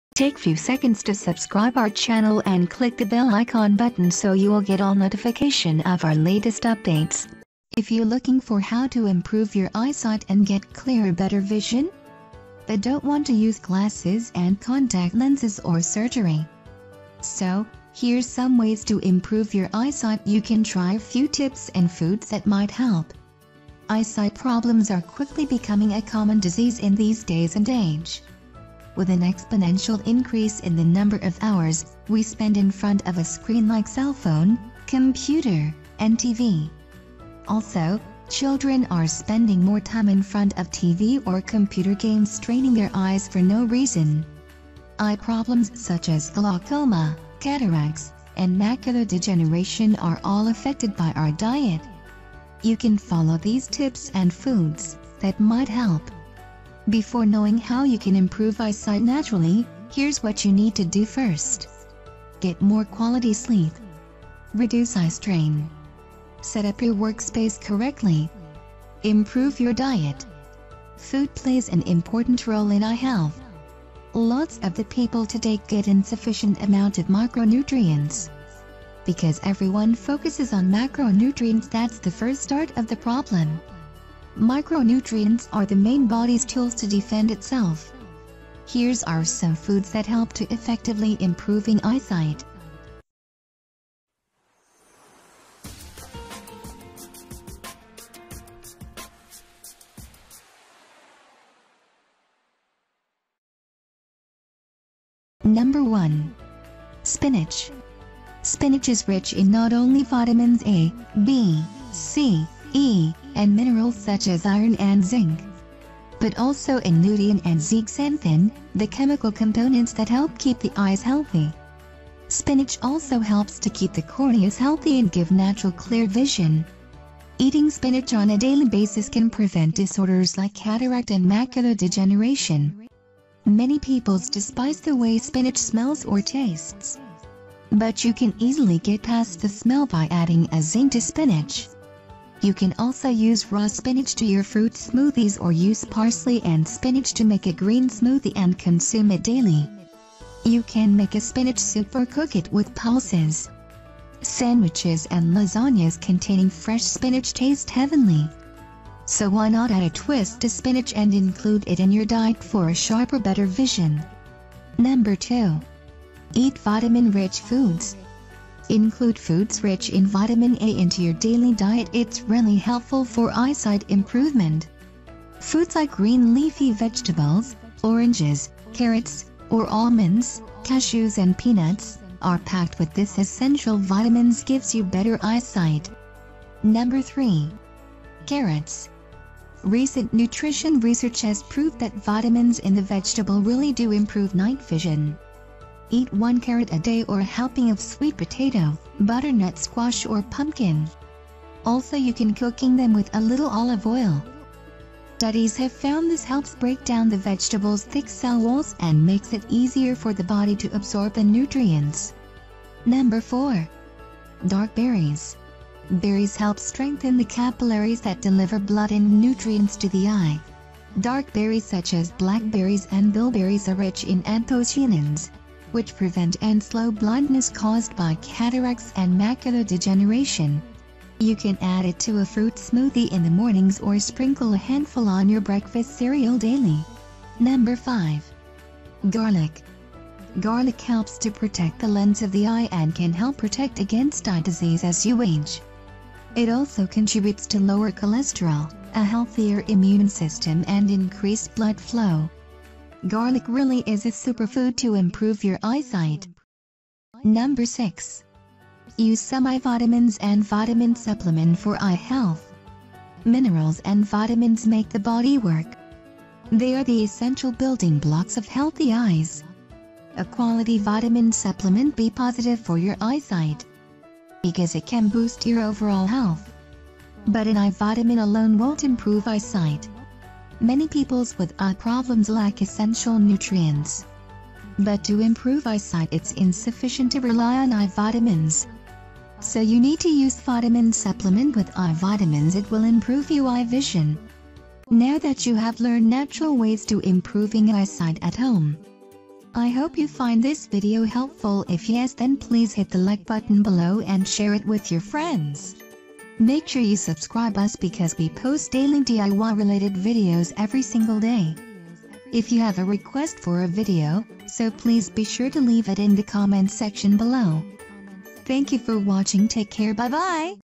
How to Improve Your Eyesight Without Glasses: Get Better Vision Naturally
If you were looking for How to Improve Your Eyesight Without Glasses and Get Better Vision Naturally?  But don't want to use glasses and contact lenses or surgery.

So,  here's some ways to improve your eyesight you can try a few tips and foods that might help.

Eyesight problems are quickly becoming a common disease in these days and age.

With an exponential increase in the number of hours,  we spend in front of a screen like a cell phone, computer, and tv. Also, children are spending more time in front of the tv or computer games straining their eyes for no reason. Eye problems such as glaucoma cataracts and macular degeneration are all affected by our diet.

You can follow these tips & foods, that might help to improve your eyesight without glasses.

How To Get Rid Of Cold Sore Naturally Fast | Cold Sore Remedies | Natural Cold Sores Treatment

Social Profiles [ Follow Us ]

Before knowing how you can improve eyesight naturally, here’s what you need to do first.

Get more quality sleep.

Reduce eyestrain.

Set up your workspace correctly.

Improve your diet.

Food plays an essential role in eye health.

Lot's of the people today get an insufficient amount of micronutrients.

Because everyone focuses on macronutrients that's the first start of the problem.

Micronutrients are the main body's tools to defend itself.

Here's are some foods that help to improve eyesight effectively.

1. Spinach.

2. Eat vitamin rich foods.

3. Carrots.

4. Dark berries.

5. Garlic.

6. Use some eye vitamins & vitamin supplement for eye health.

So you need to use vitamin supplement with eye vitamins it will improve your eye vision.

Now that you have learned natural ways of improving eyesight at home.

If you have a request for a video, so please be sure to leave it in the comment section below.

Also, Some images downloaded from shutterstock.com